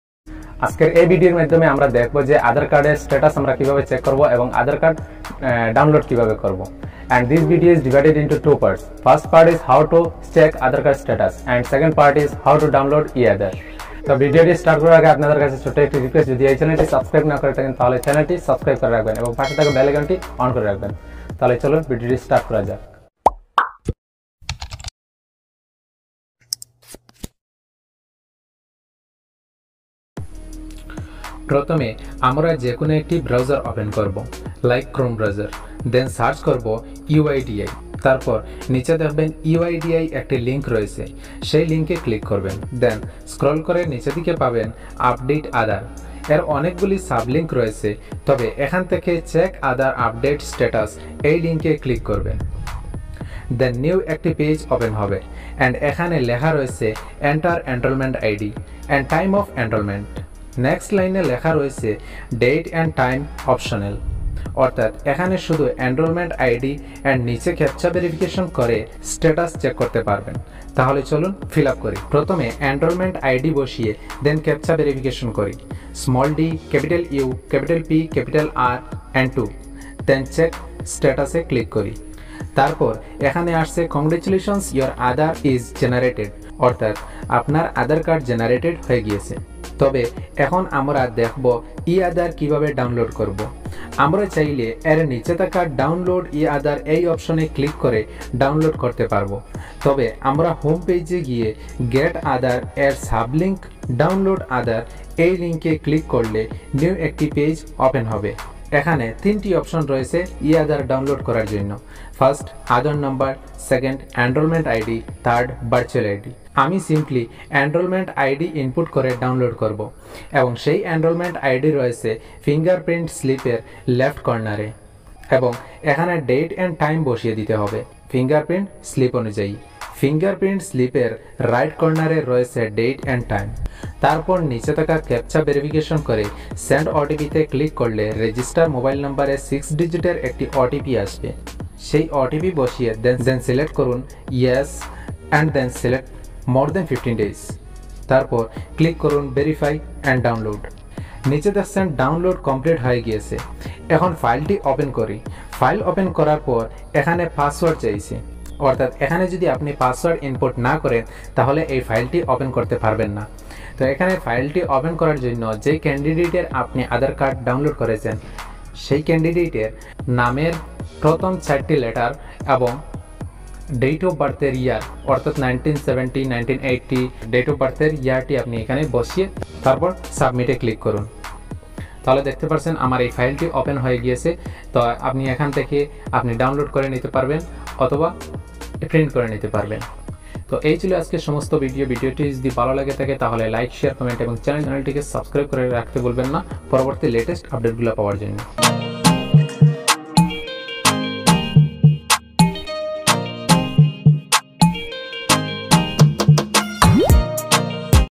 How To Check Aadhaar Status And How To Download E-Aadhaar || Y Tech Club
Full Explain about the Aadhaar status check and E-Aadhaar Download

And this is divided into two parts:-
1st How to check Aadhaar Status
2nd How to Download Aadhaar

Will get more videos to
       AND
to this "Y TECH CLUB" YouTube channel